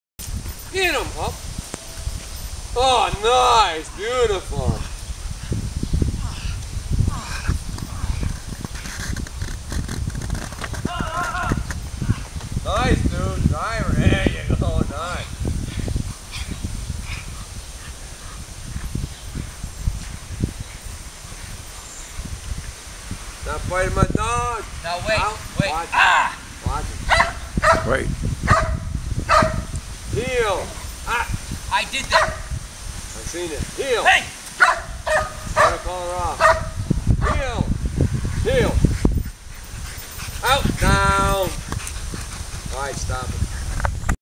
My Girl Valentine, German Shepard Doing Protection Work HARD :)
At Macs-k9 there is only one way to bite, hard and fast. Valentine is 4 years old totally trained dog that not only can be a killer, but a friend with the right command. I teach whatever my clients need. From basic , advanced , full protection, and even police training.

Let me know if you need any help with your dog Macs-k9.com